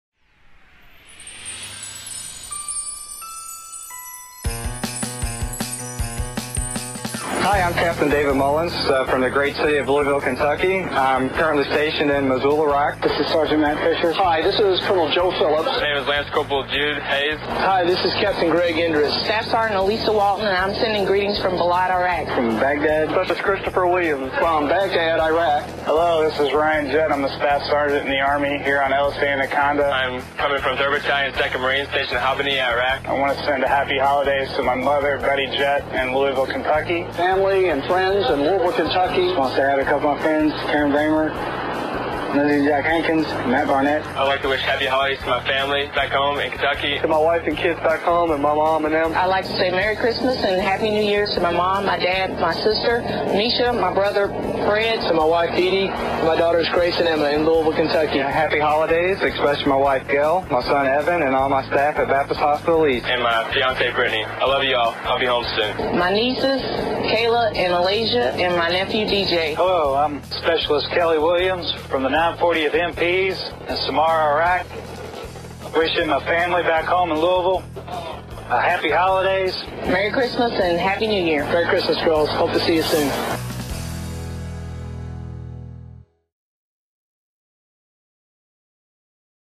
Merry Xmas From The Troops
Soldiers say hello to family and friends in Louisville